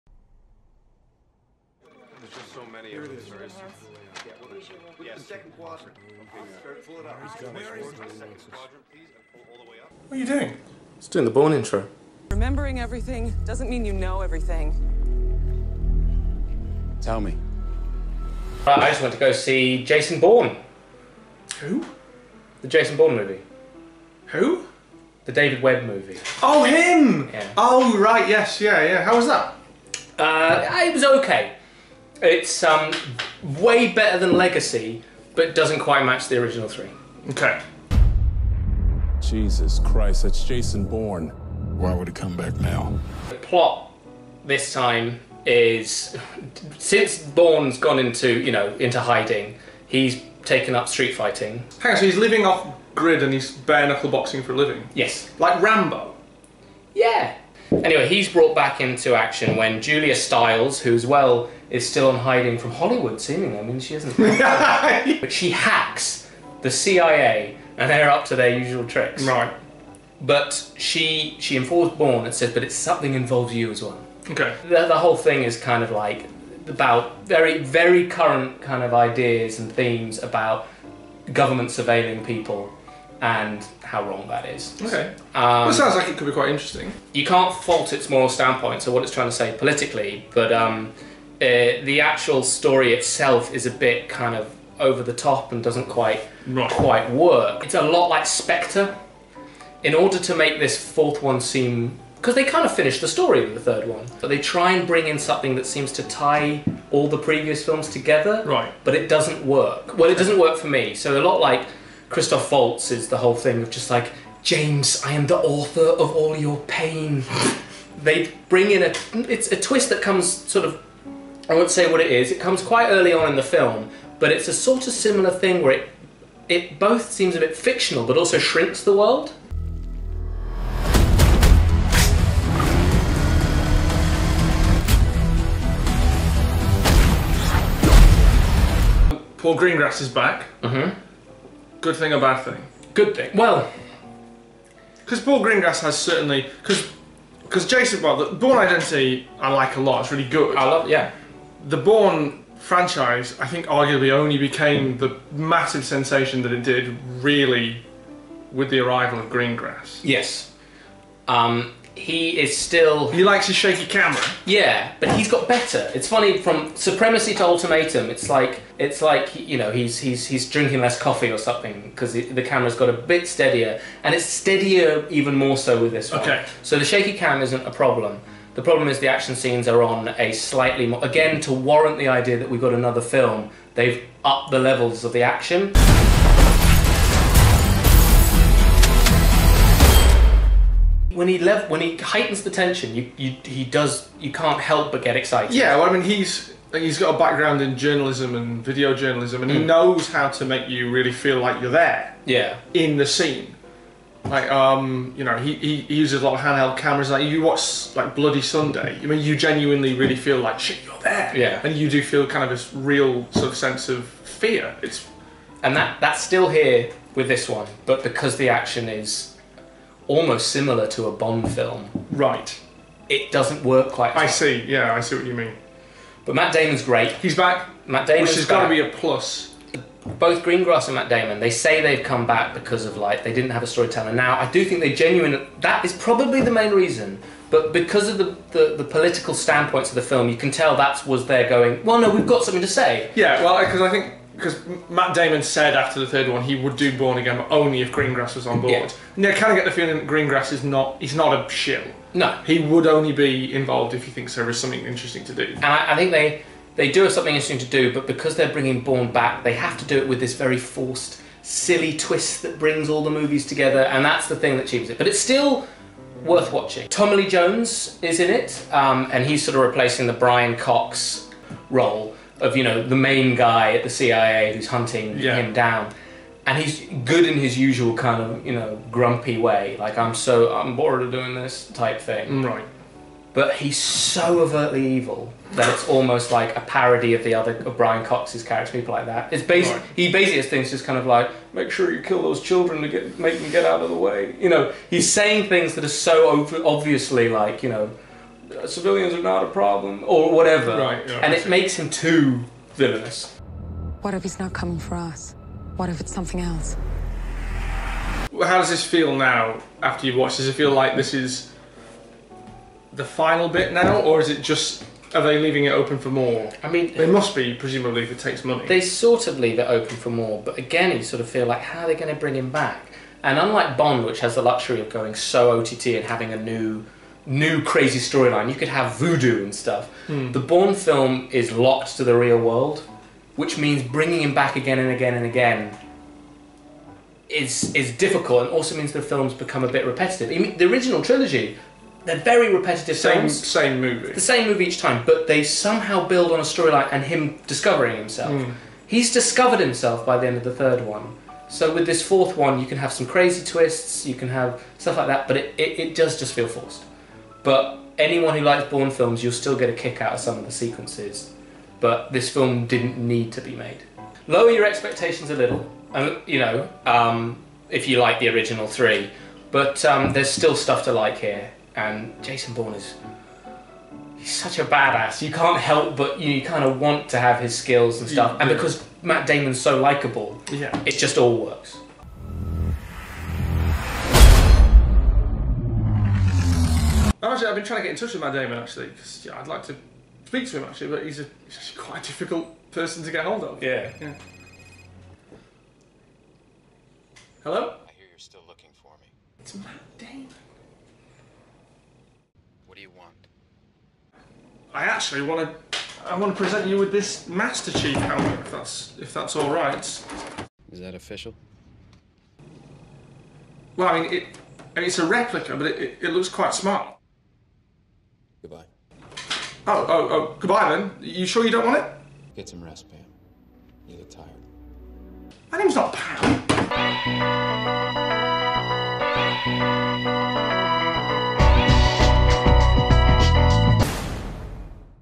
Jason Bourne Review - Spoiler Free
Harry and Chris review Jason Bourne. The 5th in the spy franchise.